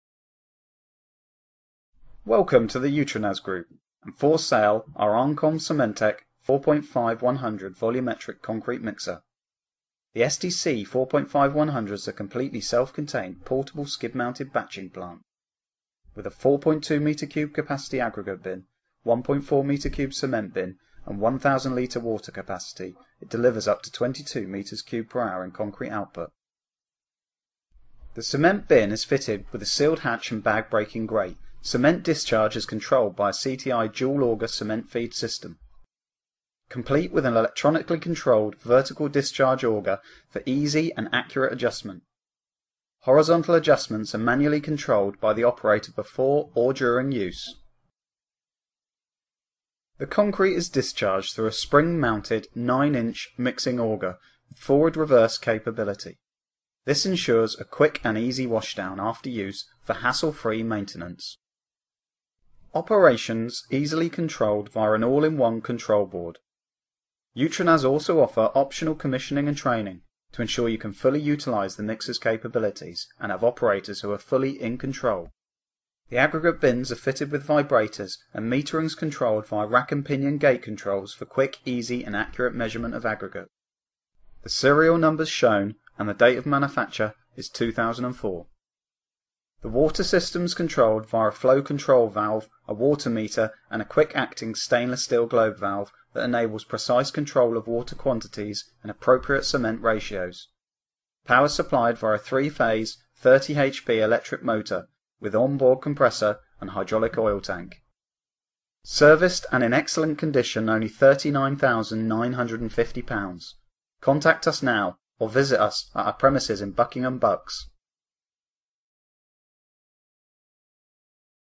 Volumetric Concrete Mixer - Armcon CemenTech SDC4.5-100
The SDC4.5-100 is a completely self-contained, portable, skid-mounted concrete batching plant. With a 4.2m capacity aggregate bin, 1.4m cement bin, 1000 litre water tank capacity delivering up to 22m3/hour concrete output. Power supplied via a 3 Phase (200/415v) 30 HP electric motor with on-board compressor and hydraulic oil tank. Aggregate bins are fitted with vibrators for accurate measurement of aggregates. Water system controlled via flow control valve, water meter and quick-acting stainless steel globe valve that enables precise control of water quantities and appropriate cement ratios. Cement bin fitted with sealed hatch and bag breaking grate, cement discharge controlled by CTI dual auger cement feed system. Concrete discharged through a spring mounted 9" mixing auger with forward / reverse capacity for quick and easy wash-down. Serial no. 45SCD301580EH. Date of manufacture 2004. Serviced and in excellent condition.

UTZ35B41429

Price: £39,950